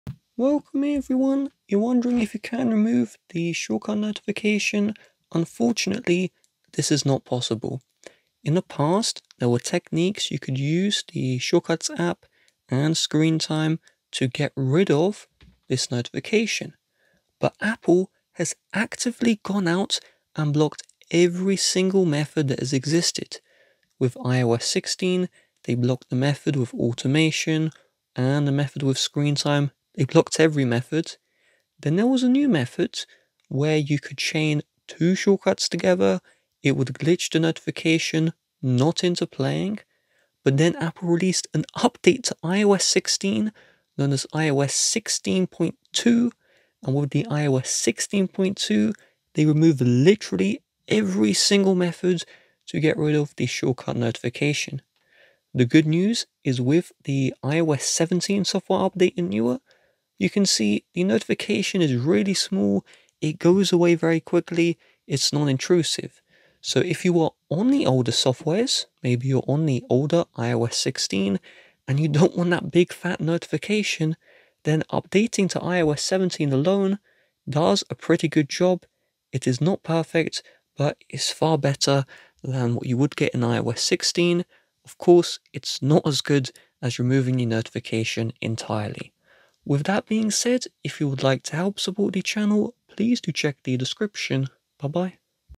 Can You Remove Shortcuts Notification on iPhone iOS 17? (no)
Learn why it is not possible to remove Shortcuts Notification on iPhone with the iOS 17 software update. Also learn why it was possible to remove the shortcut notification with the older iOS software, and what Apple has been doing to put a stop to that.